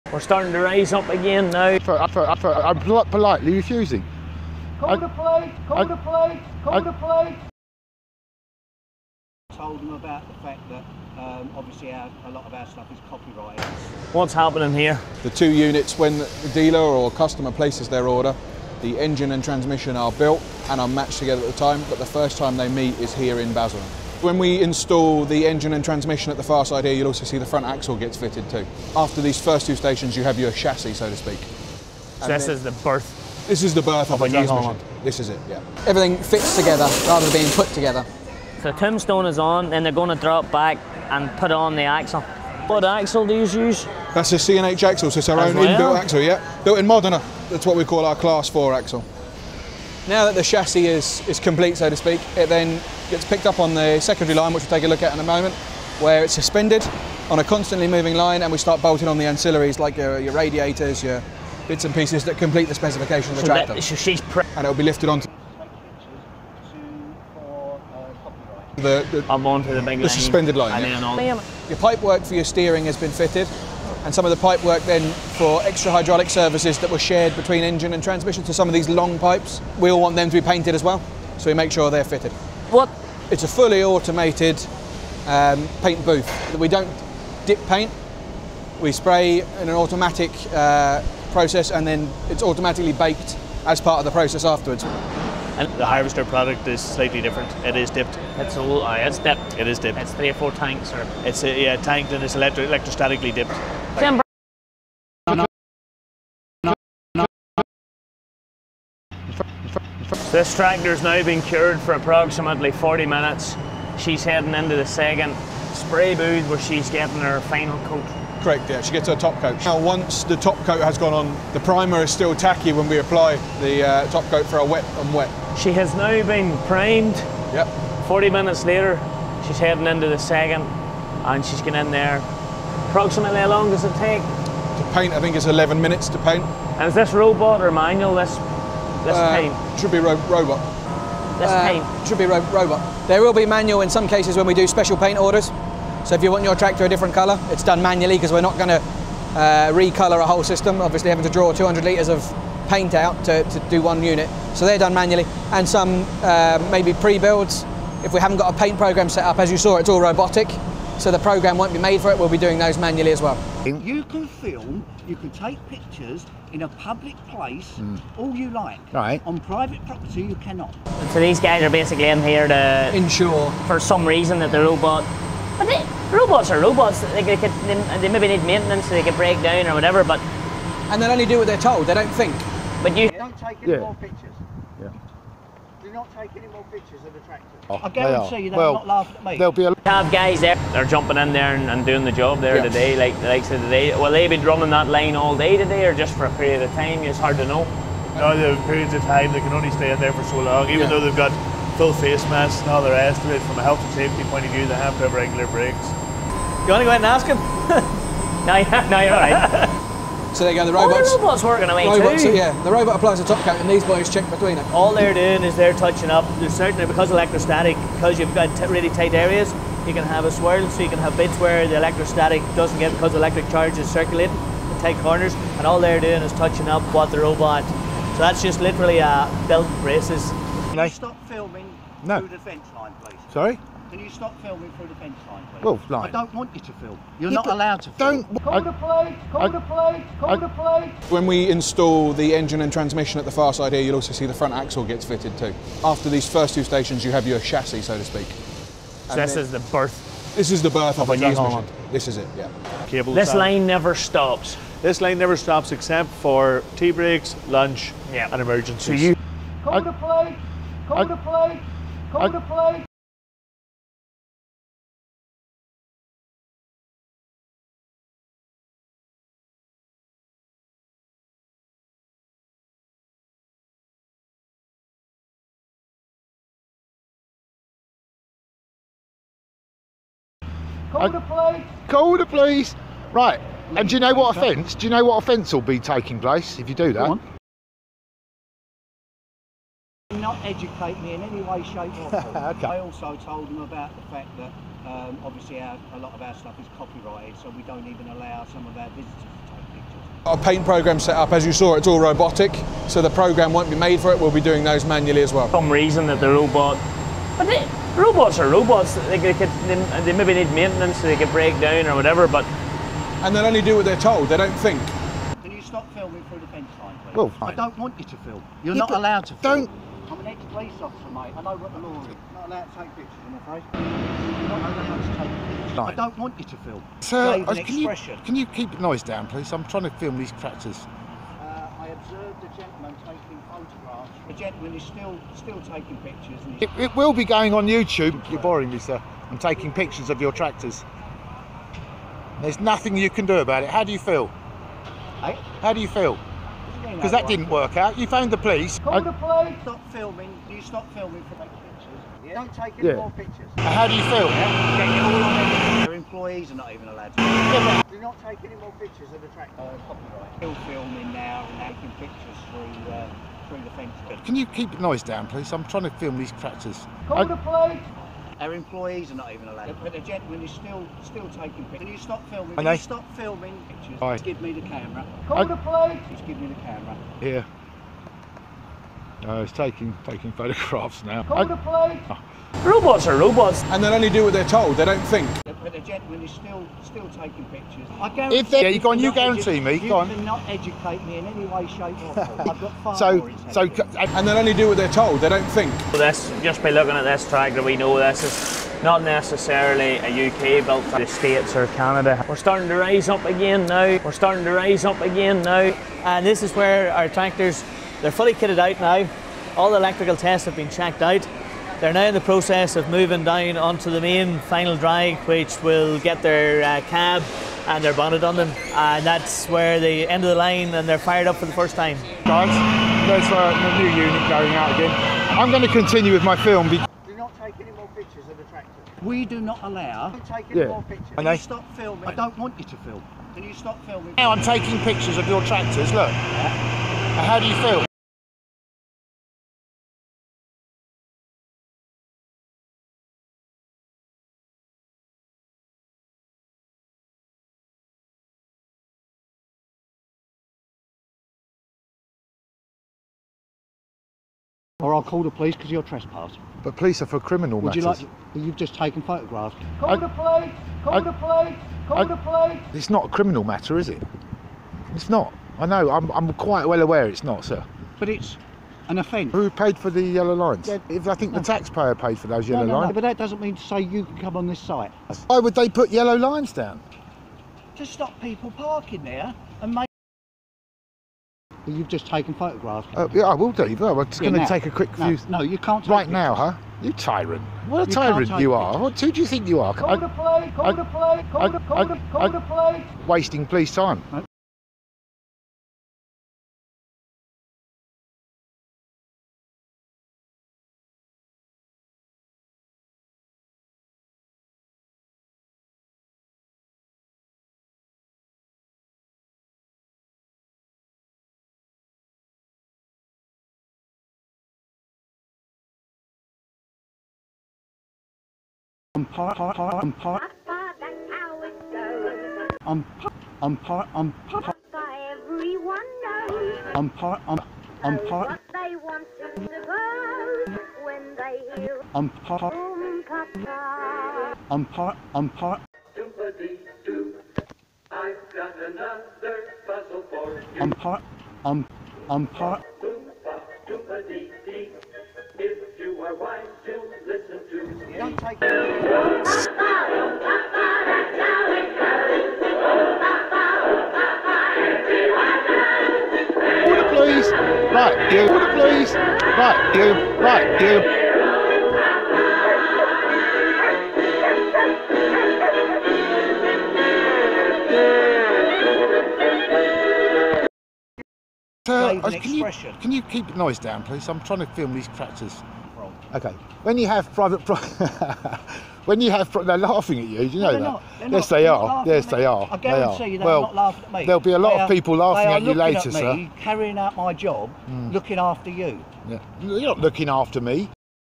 CNH New Holland Agriculture..*CALL THE POLICE!! *
In this film, between quite interesting & a mackerel security guard on security, myself & Kevin the Twatctor identify a common social injustice.
That we pay for, but don't get, our own police force!
Because they're always occupied looking after themselves & the super-rich!
Its CNH Tractor Factory in Basildon in Essex .
We are AUDITING. The best out there for me, Auditing Britain, Focus Pocus,Marti Blagborough, DJ Audits, PJ Audits & many more , using camera's to check our rights!